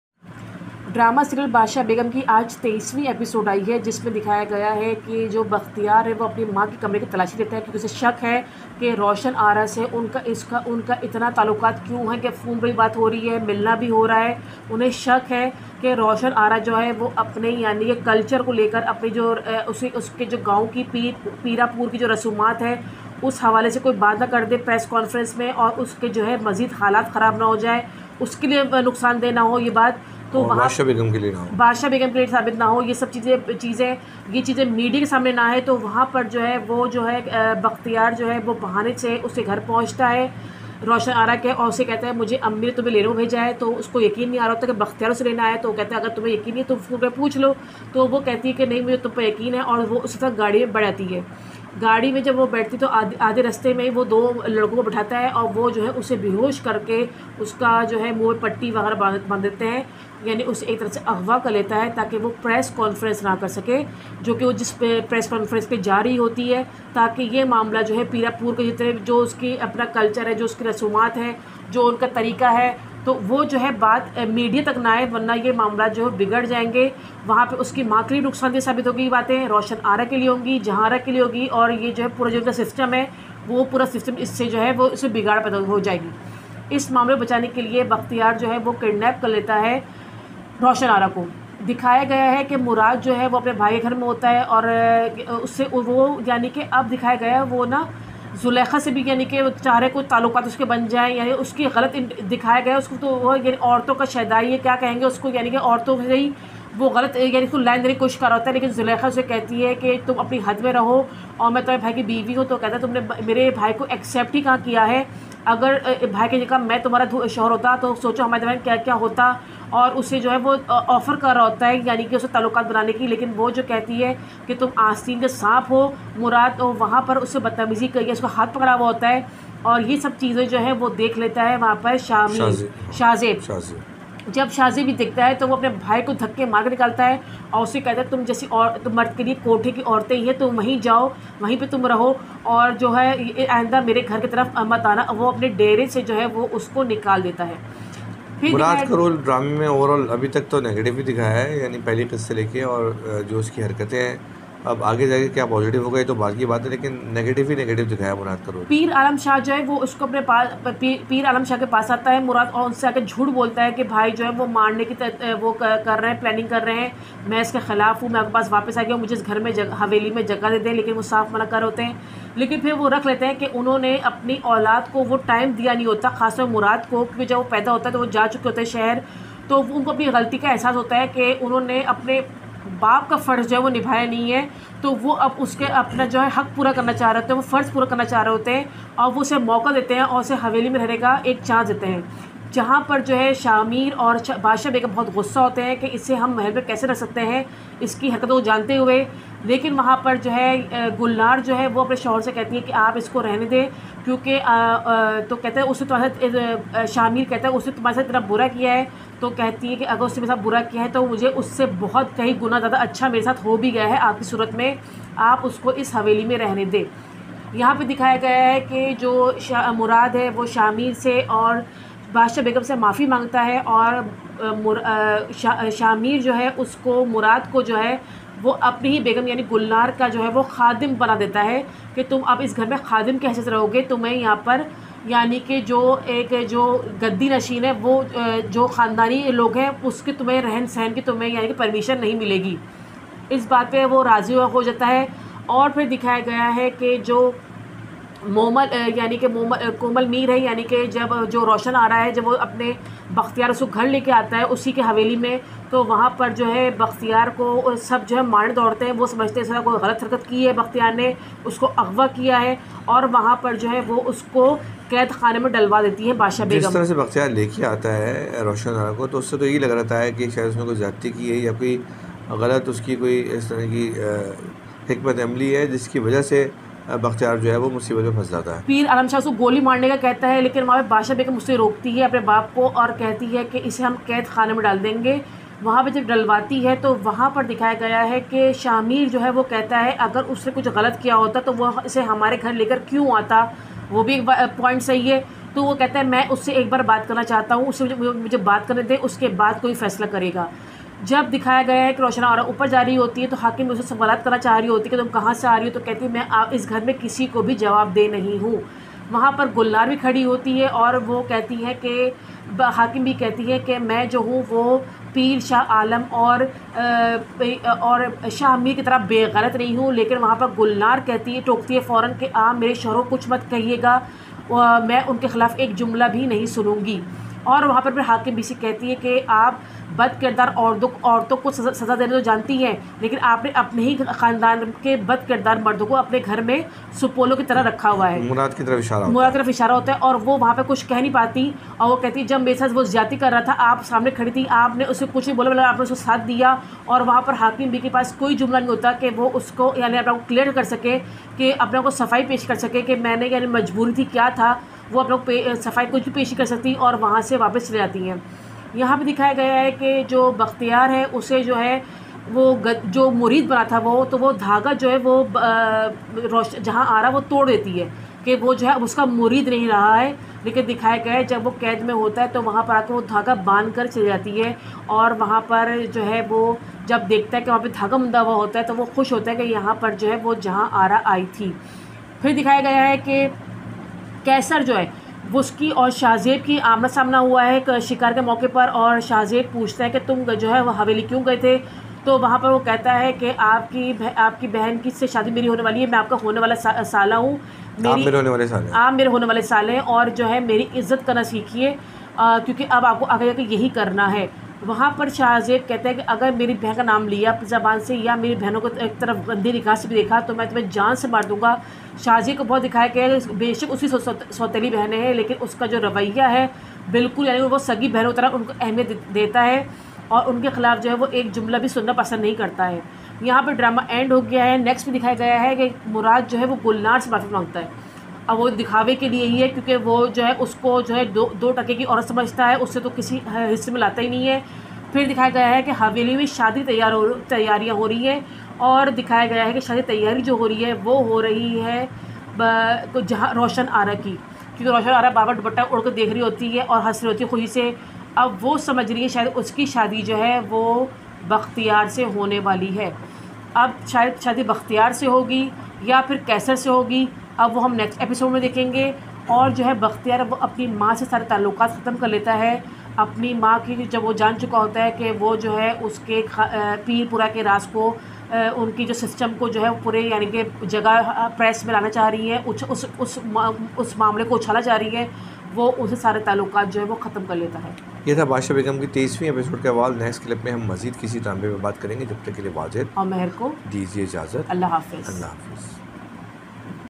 wajid isfahani and mehar wajid channel | review drama badshah begum episode 23 | 23 aug 22
wajid isfahani and mehar wajid channel , review drama badshah begum episode 23rd , 23 august 22.
Review of the episode 23rd of drama Badshah Begum on Wajid Isfahani and Mehar Wajid channel.
Shah Mir and Badshah Begum make Murad a servant of Gulnar. Bakhtiar brings Roshan Ara back to her mansion. Hakim B asks Roshan Ara what she said. Roshan Ara says she is not answerable to anyone.